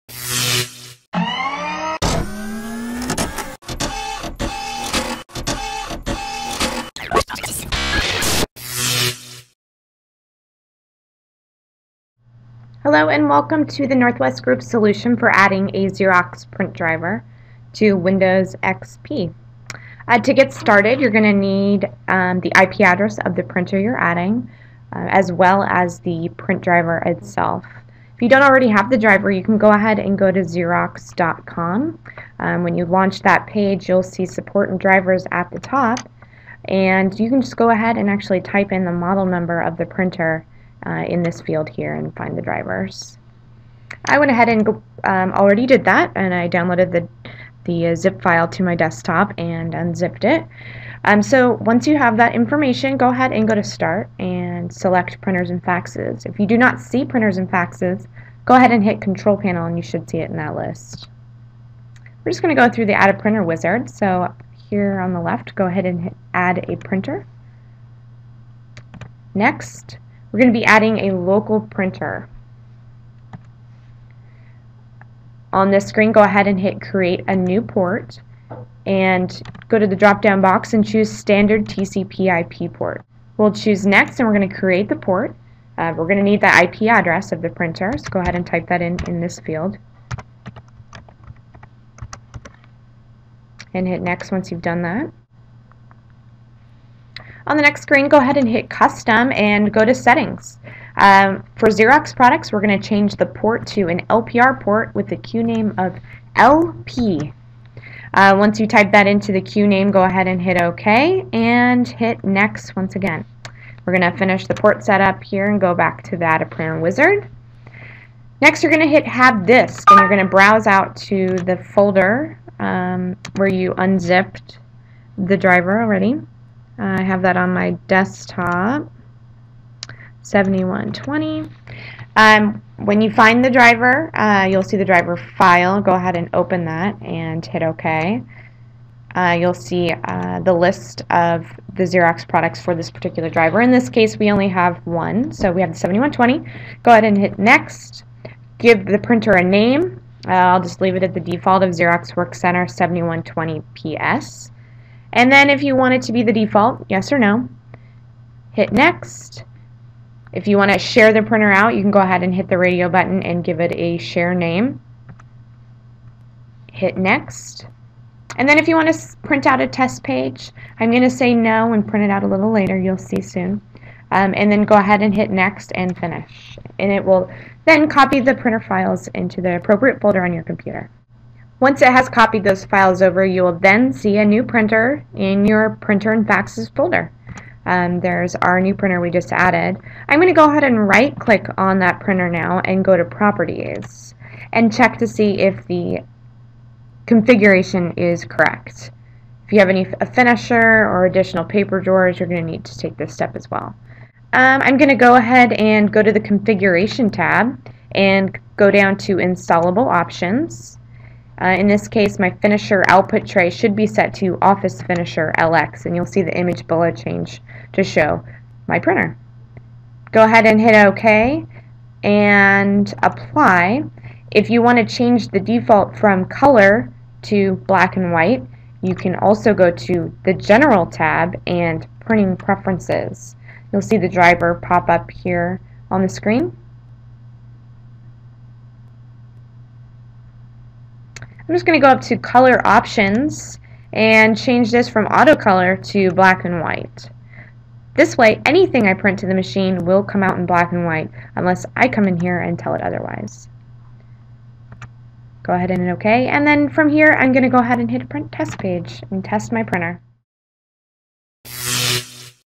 How to Add a Xerox Print Driver
A Northwest Group solution video on how to add a Xerox print driver to Windows XP.